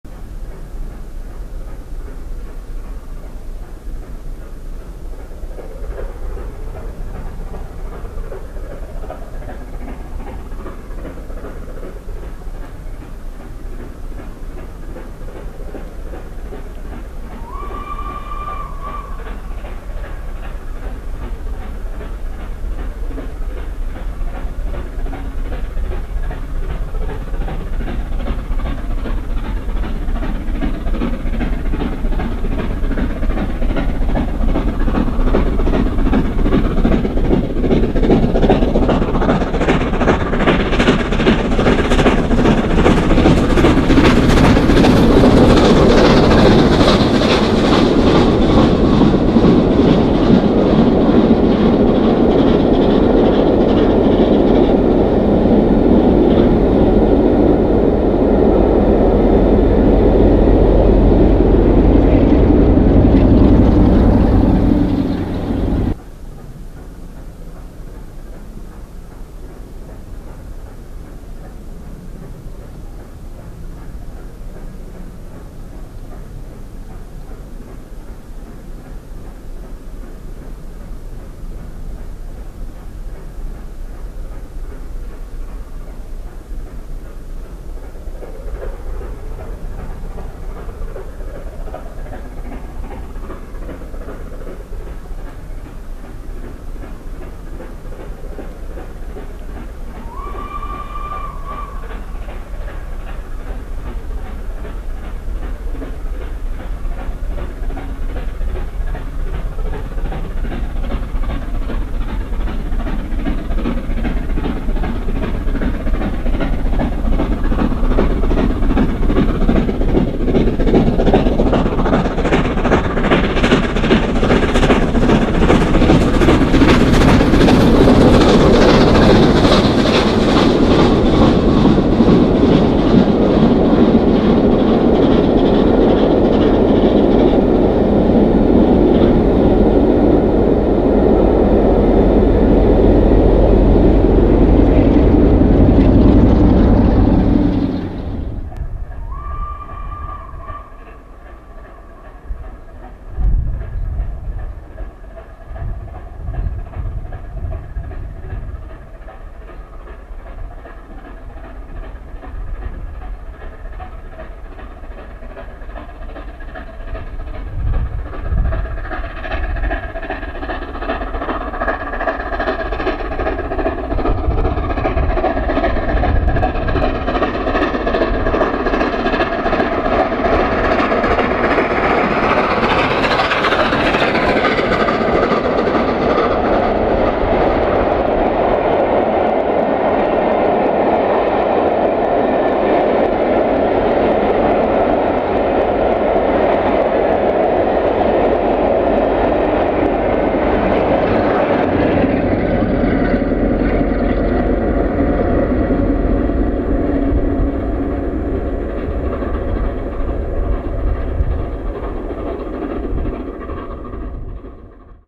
LNER 60103 Shines in the Sun on the Hadrian 14/10/23.
LNER A3 60103 Flying Scotsman was the motive power for the Hadrian tour that started at Derby with 60103 joining the tour from Hellifield . A brisk windy chilly morning sees 60103 at Selside Cottages but sadly wrong side for the sun and then at Selside  Shaw . Thanks to Nige for the company .